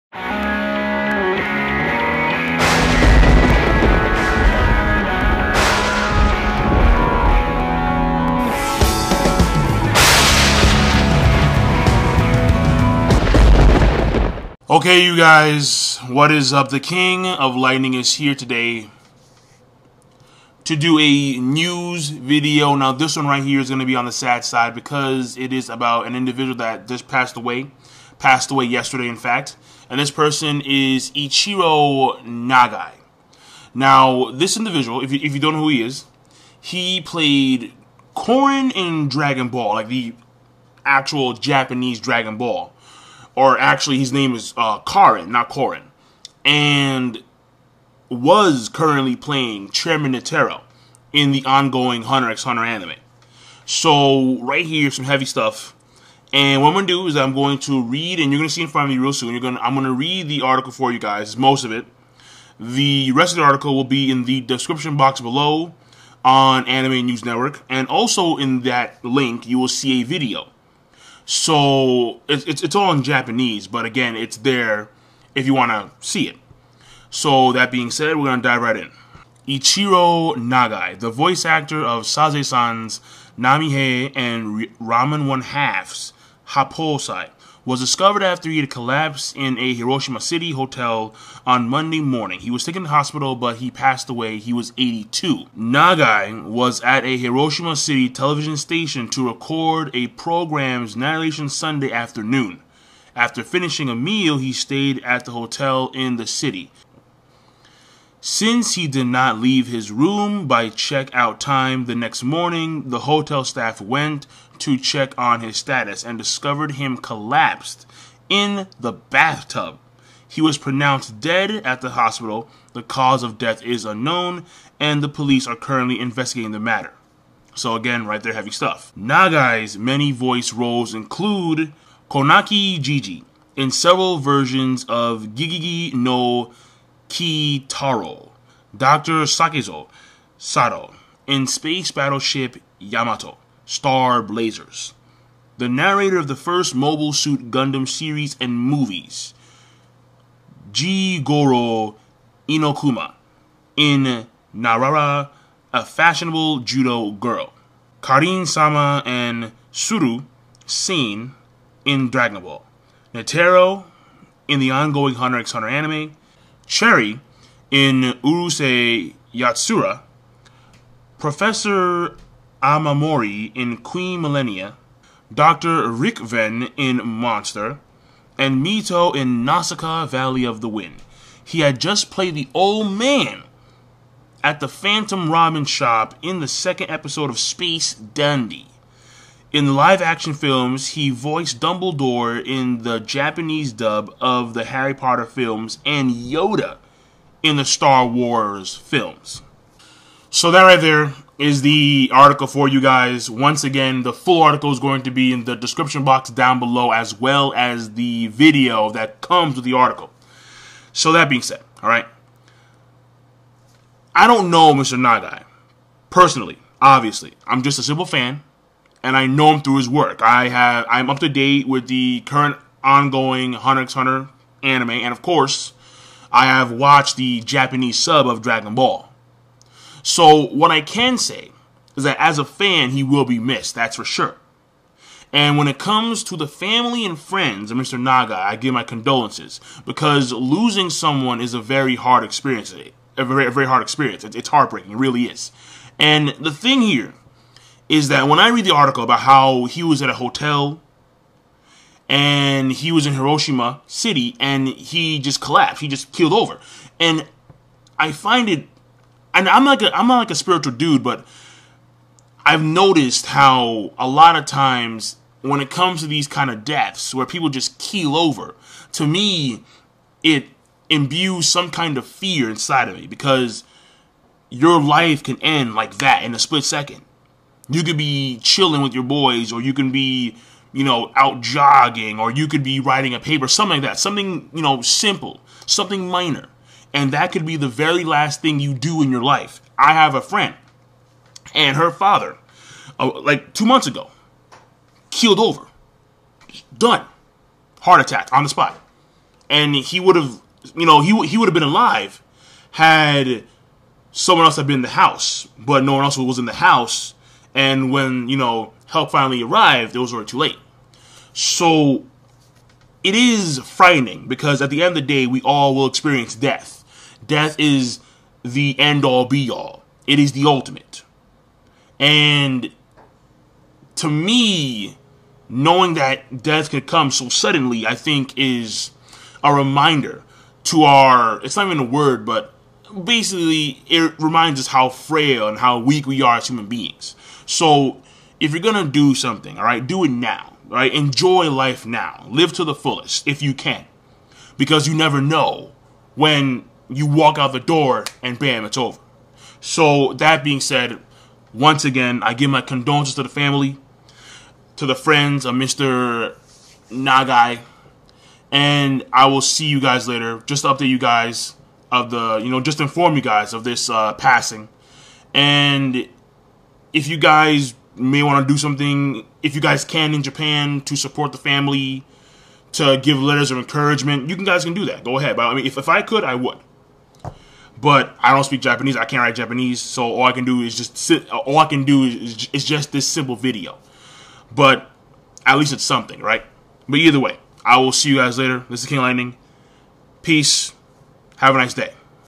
Dragon Ball's Korin Voice Actor Ichirô Nagai Passes Away At 82
Prominent individual in Japan Voice Acting Passes Away.